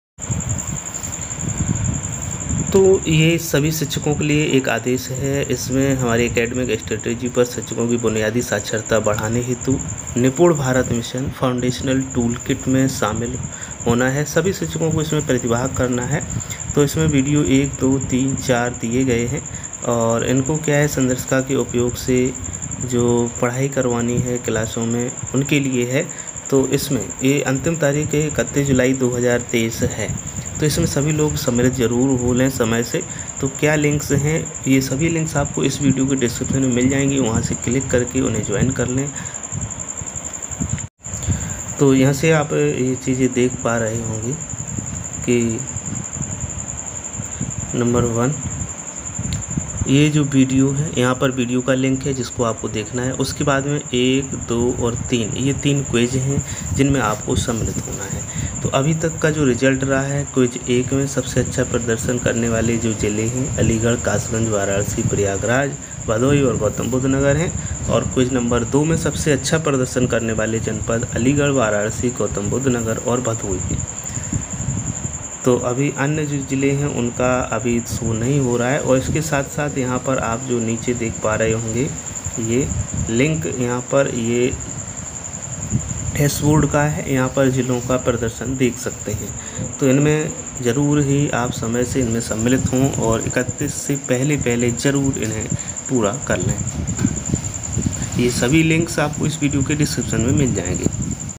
निपुण भारत मिशन के अंतर्गत 2023-24 की एकेडमिक स्ट्रेटज आधारित Quiz के लिंक्स
निपुण भारत मिशन के अंतर्गत 2023-24 की एकेडमिक स्ट्रेटजी को शिक्षकों को समझाने के लिए विभाग द्वारा *NIPUN Bharat Foundational Toolkit Youtube* के माध्यम से साझा की जा रही है । इस टूलकिट पर आधारित *Quiz No. 2* सभी शिक्षकों के साथ साझा किया जा रहा है।

*Quiz No.2  में सबसे अच्छा प्रदर्शन करने वाला जनपद :* अलीगढ़, वाराणसी, गौतम बुद्ध नगर व भदोही 👏🙌
*Quiz No.1  में सबसे अच्छा प्रदर्शन करने वाला जनपद :* अलीगढ़, कासगंज, वाराणसी, प्रयागराज, भदोही व गौतम बुद्ध नगर 👏🙌

*शिक्षक प्रशिक्षण की जनपदवार वर्तमान स्थिति dashboard पर प्रदर्शित की गई है। सभी शिक्षक इस toolkit का अध्ययन कर Quiz No. 1 व 2 में प्रतिभाग अवश्य करें।*

धन्यवाद ।